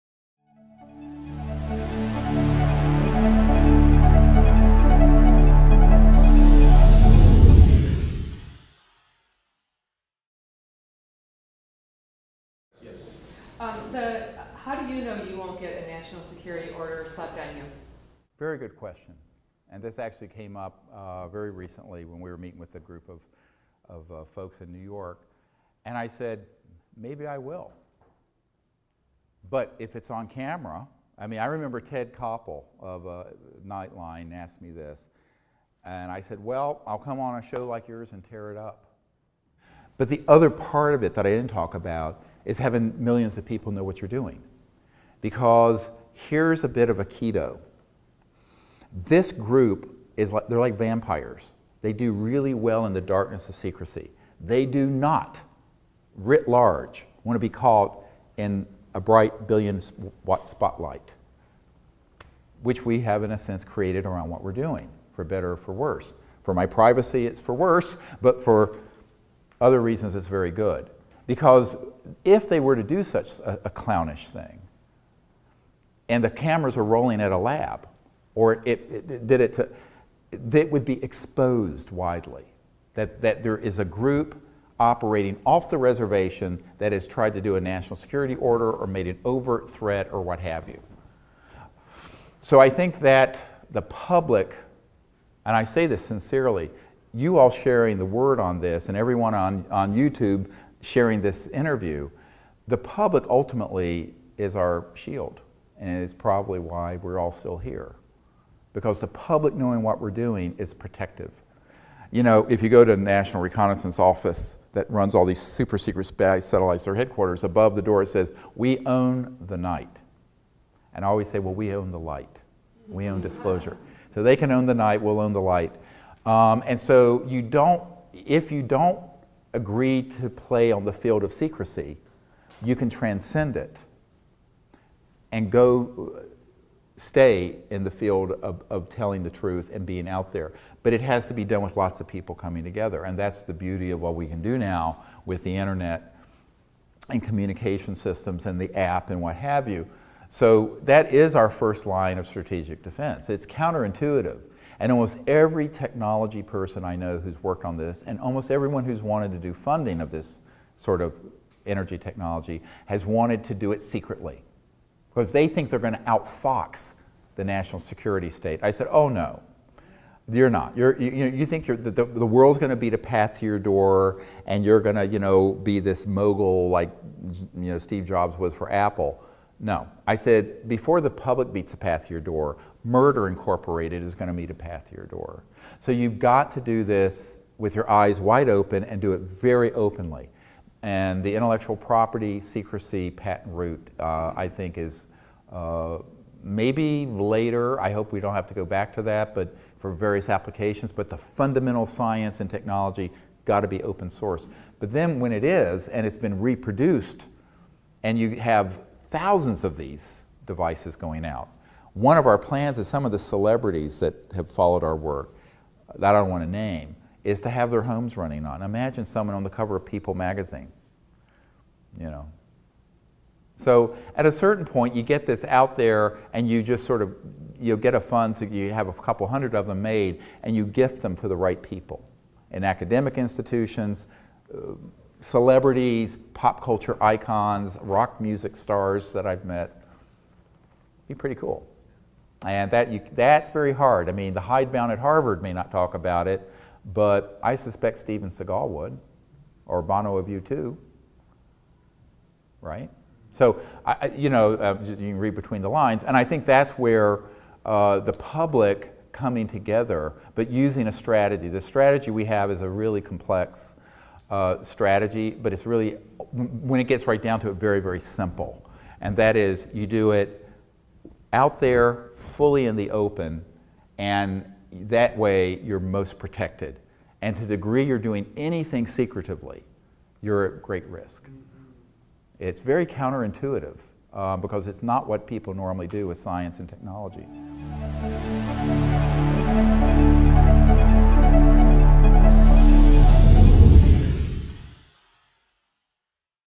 2014-04-13 | Dr. Greer's Introduction to Free Energy | Part 11 - Q&A: NSO National Security Order
Listen to Dr. Steven Greer's overview of the New Energy movement: devices, inventors, suppression, strategy. What has not worked and why and what will work to bring out energy systems that will change the world.